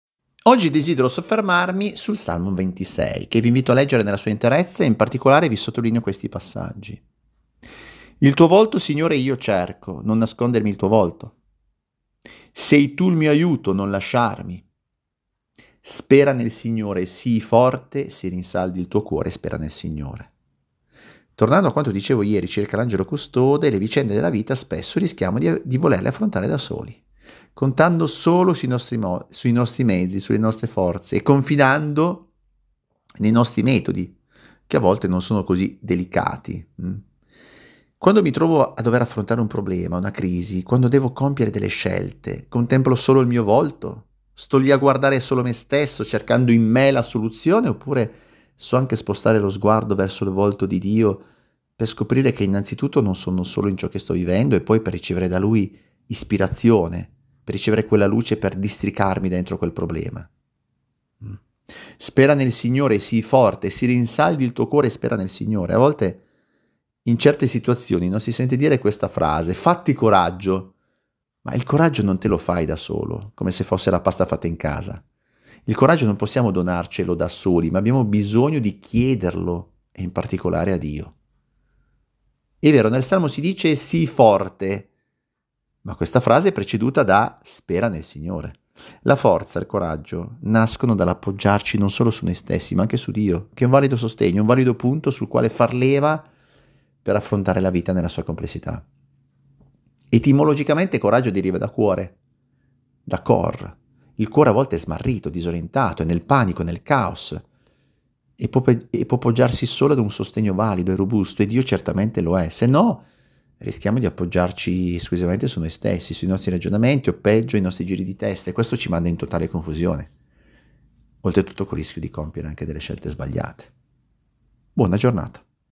Il tuo volto, Signore, io cerco - don Paolo Quattrone
Commento di don Paolo Quattrone - sacerdote della diocesi di Aosta, parroco di Hône e Bard.
Giovedì 3 ottobre

Oggi desidero soffermarmi sul salmo 26 che vi invito a leggere nella sua interezza e in particolare vi sottolineo questi passaggi: Il tuo volto, Signore, io cerco. Non nascondermi il tuo volto,… Sei tu il mio aiuto, non lasciarmi… Spera nel Signore, sii forte, si rinsaldi il tuo cuore e spera nel Signore. Tornando a quanto dicevo ieri circa l’angelo custode, le vicende della vita spesso rischiamo di affrontarle da soli, contando solo sulle nostre forze e confidando nei nostri metodi che a volte non sono così delicati. Quando mi trovo a dover affrontare un problema, una crisi, quando devo compiere delle scelte contemplo solo il mio volto, sto lì a guardare solo me stesso cercando in me la soluzione oppure so anche spostare lo sguardo verso il volto di Dio per scoprire che innanzitutto non sono solo in ciò che vivo e poi per ricevere da Lui ispirazione, luce per districarmi dentro quel problema? Spera nel Signore, sii forte, si rinsaldi il tuo cuore e spera nel Signore. A volte in certe situazioni si sente dire questa frase: “fatti coraggio”. Il coraggio non te lo fai da solo come se fosse la pasta fatta in casa, il coraggio non possiamo donarcelo da soli ma abbiamo bisogno di chiederlo e in particolare a Dio. È vero nel salmo si dice: “Sii forte” ma questa frase è preceduta da “Spera nel Signore”. La forza, il coraggio nascono dall’appoggiarci non solo su noi stessi ma anche su Dio che è un valido sostegno, un valido punto sul quale far leva per affrontare la vita nella sua complessità. Etimologicamente coraggio deriva da cuore, il cuore a volte è smarrito, disorientato, è nel panico, nel caos e può poggiarsi solo ad un sostegno valido e robusto e Dio certamente lo è sennò rischiamo di appoggiarci solo su noi stessi, sui nostri ragionamenti o peggio i nostri giri di testa e questo ci manda in totale confusione e ci fa compiere anche scelte sbagliate.